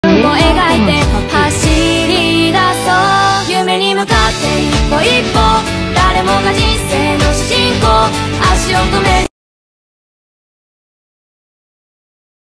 2年前と比較してみた。
【乙武義足プロジェクト】2年前から、ずいぶん進歩しました！！

クラウドファンディングも応援してください！！

《国立競技場のトラックで100メートル歩きたい！！》